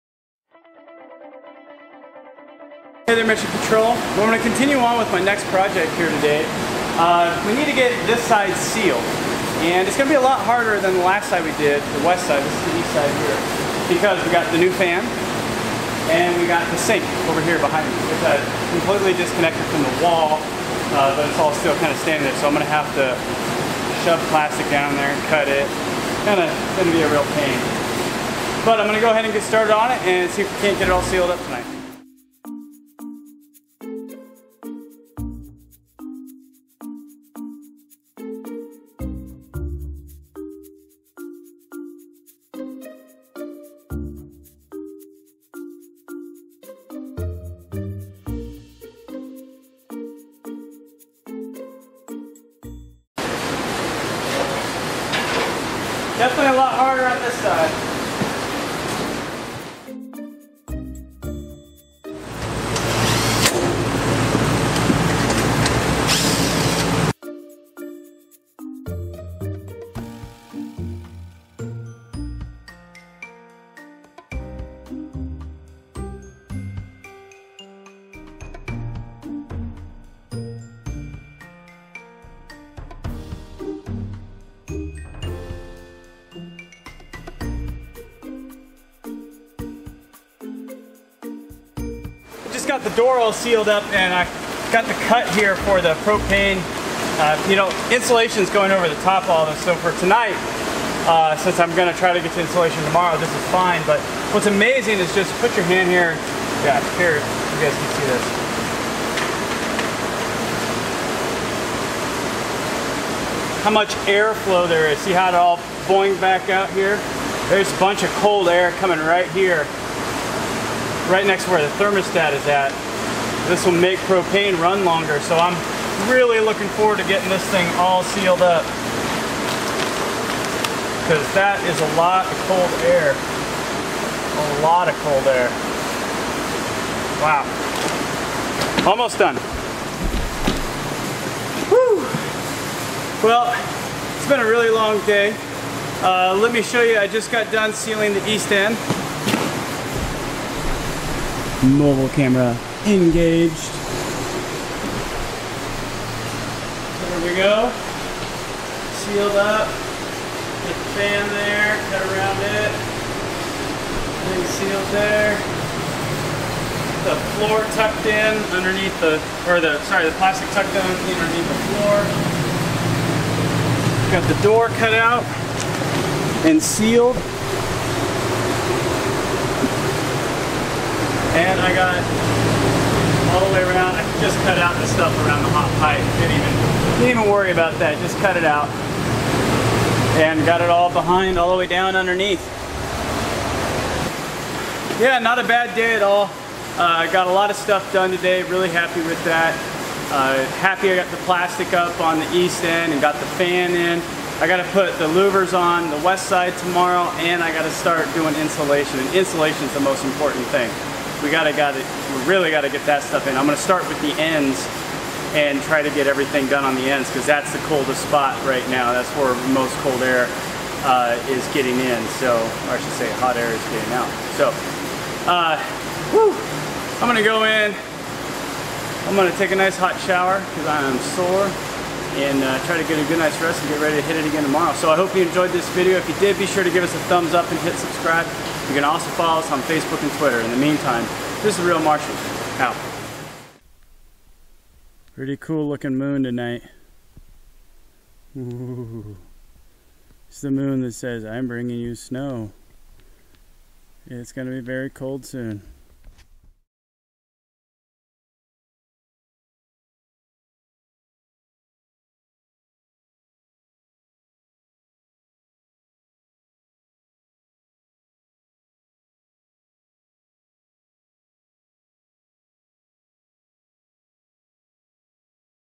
How to Insulate a GREENHOUSE - Part 2
with the new greenhouse cover on it's time to start prepping for insulation. Before I can insulate I have to seal both ends using 6mil plastic and greenhouse clips.